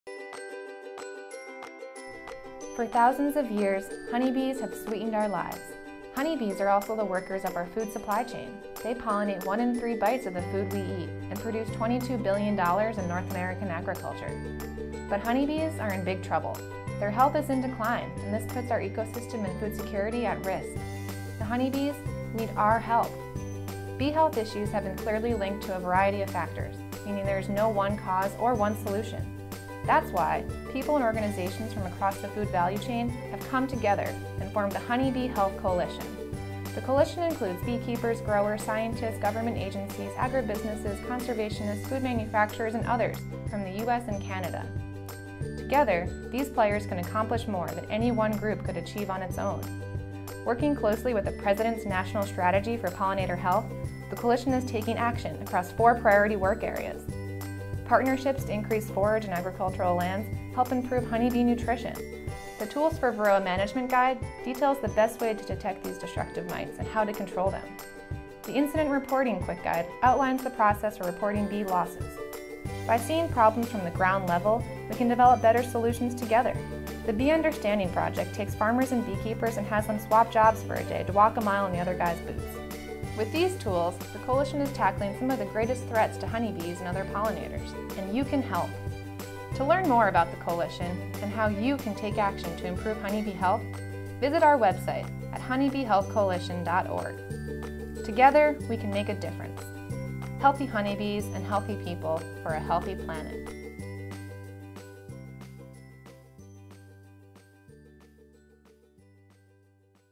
The Honey Bee Health Coalition: Achieving a Healthy Future for Pollinators
The U.S. Canola Association is a member of the Honey Bee Health Coalition. Learn more about the Coalition's goal of reversing recent declines in honey bee health and ensuring the long-term health of honey bees and other pollinators.